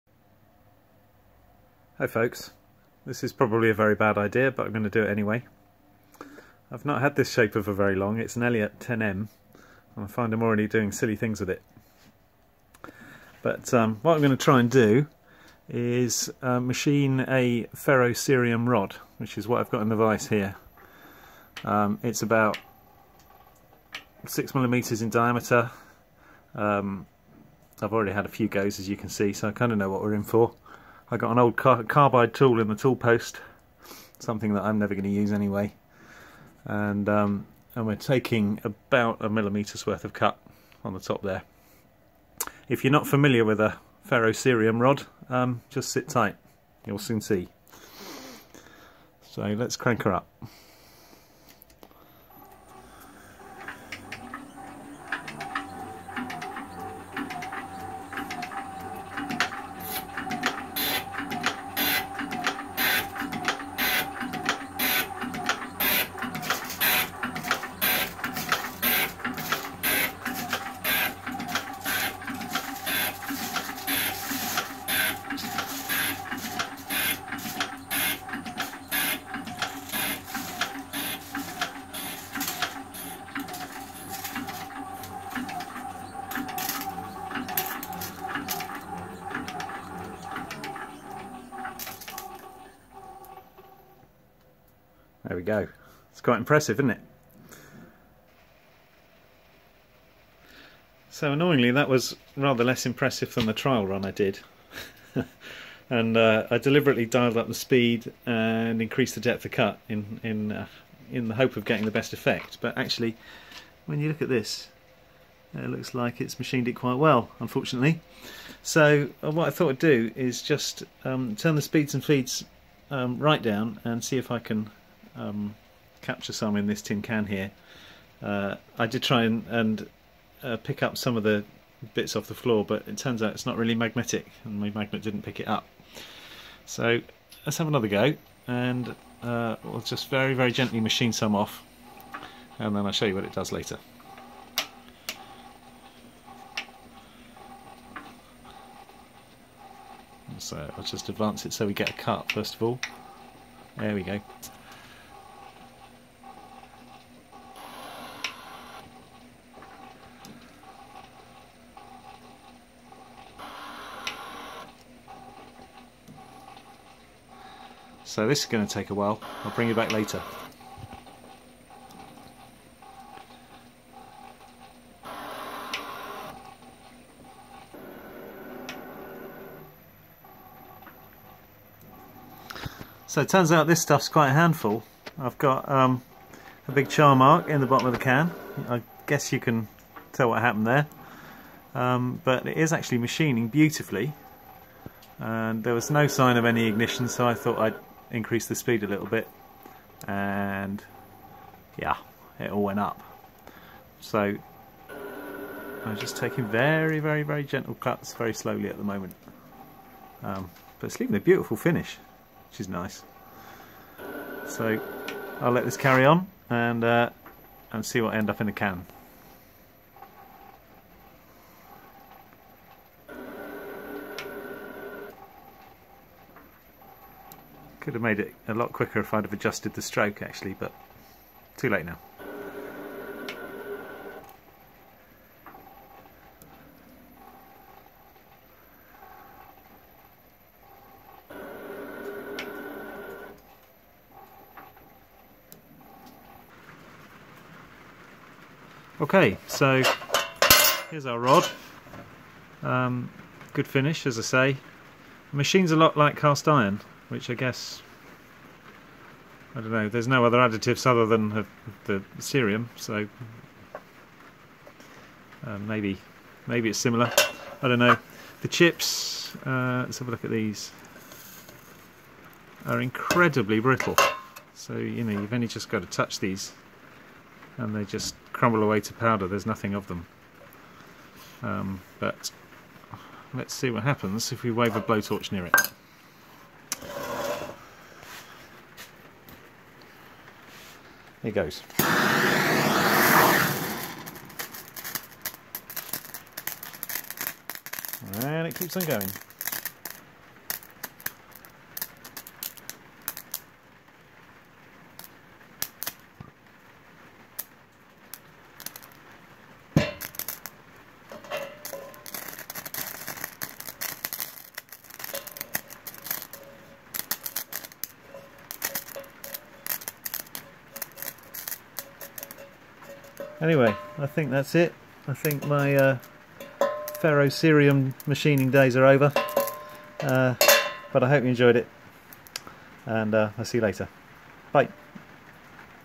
Elliott shaper machining ferrocerium
Elliott 10M shaper machining a 6mm ferrocerium rod.

Often called 'ferro rod', 'fire steel' or incorrectly 'flint'.

If the chips don't oxidize they would be an ideal fire starting aid in addition to scraping the rod itself.

Thanks to Aaron for the ferrocerium!